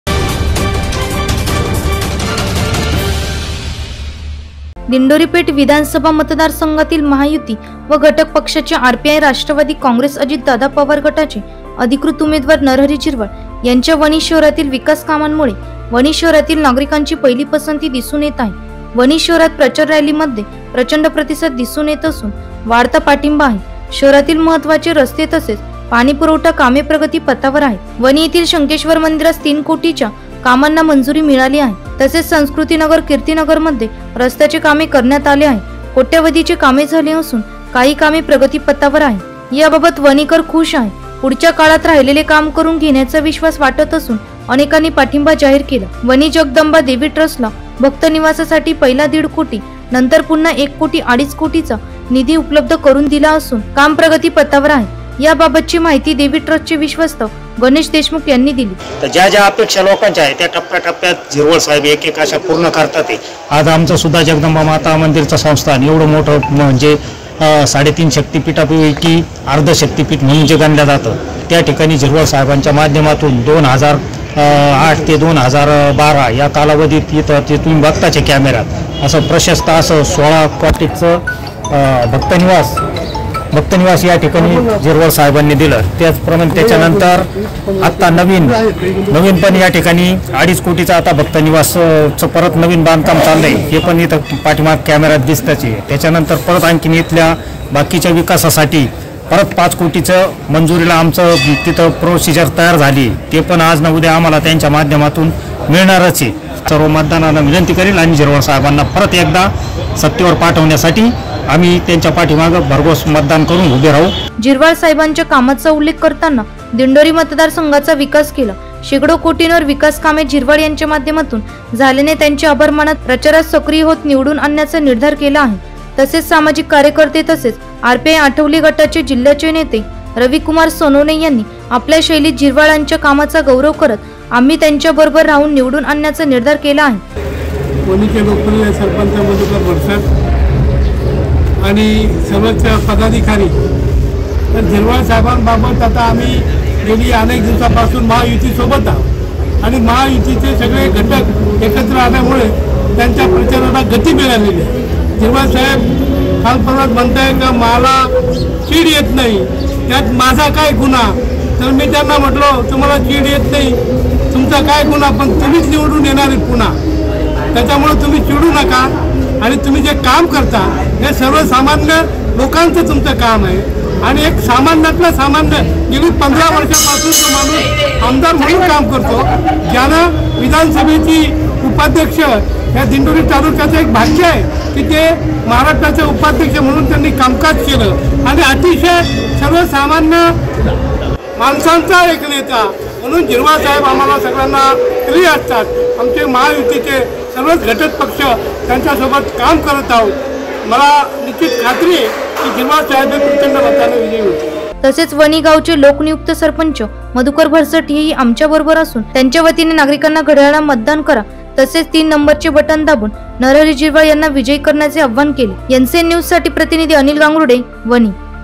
वणी । वणीतील विकासामुळे वनीकरांची महायुतीचे उमेदवार नरहरी झिरवाळ यांना पहिली पसंती
NCN News